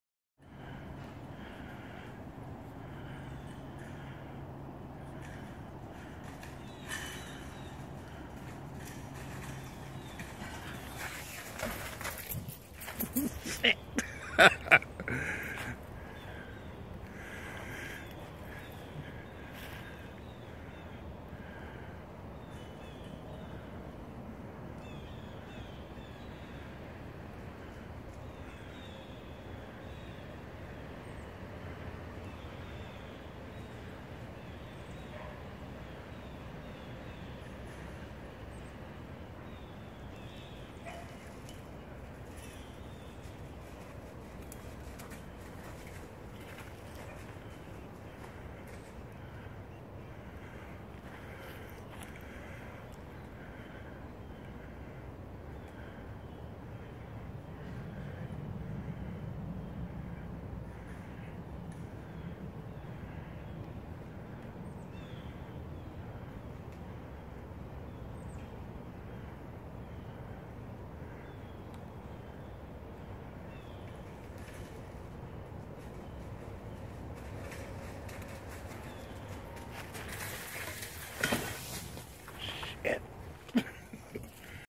Close encounter
Dear stuck inside our motorpool.